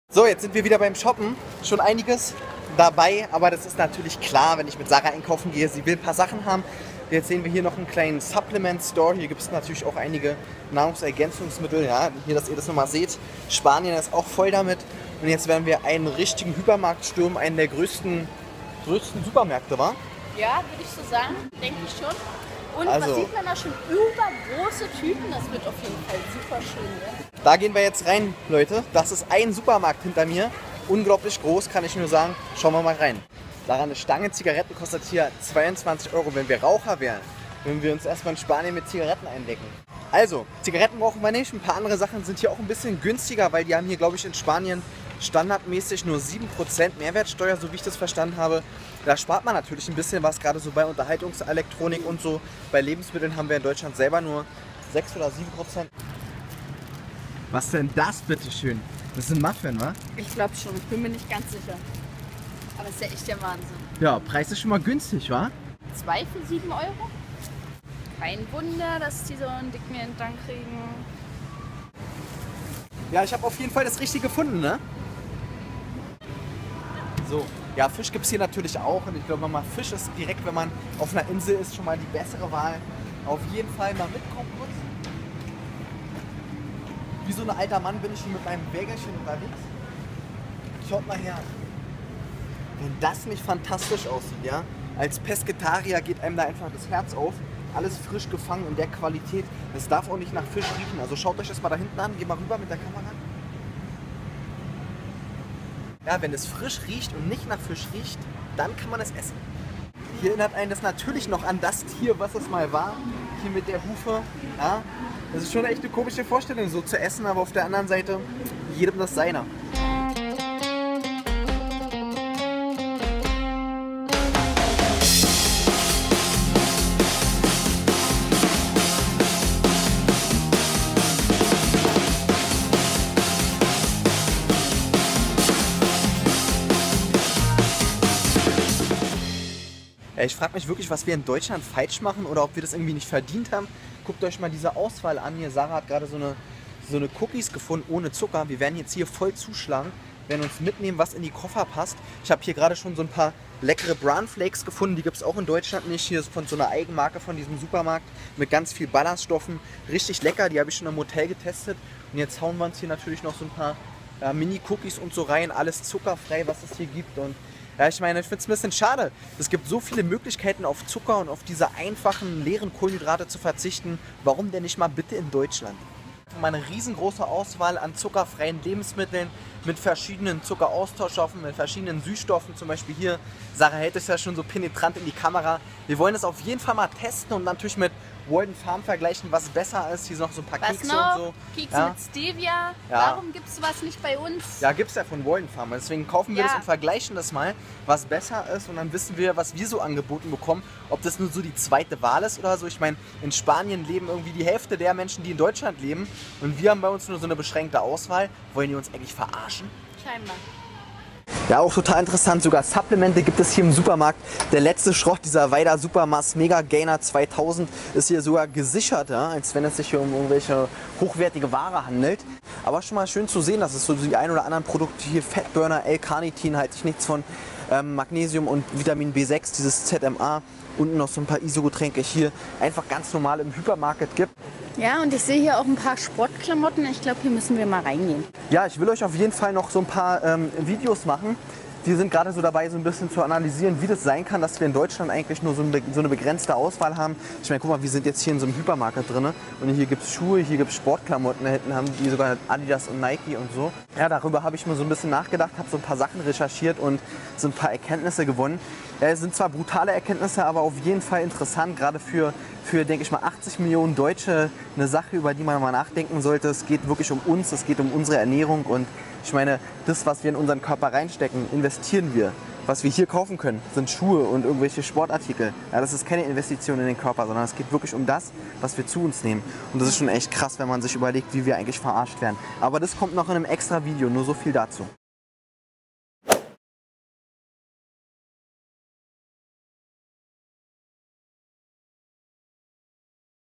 Gran Canaria Vlog #3 Einkaufen im Hypermarket - Wichtige Ankündigung
In diesem Video gehe ich in Carrefour auf Gran Canaria in Spanien einkaufen mit Sarah. Neben Lebensmitteln und viele Diätprodukten die ideal in einer Definitionsphase sind, gibt es auch Sport Bekleidung und Fitness Artikel in diesem Supermarkt - wohl eher Hypermarket.
Im nächsten Video erkläre ich Euch warum wir in Deutschland viele Produkte vorenthalten bekommen und nicht essen können!

Ich liebe diese Marke (wenn ich aus dem Gym rauskomme)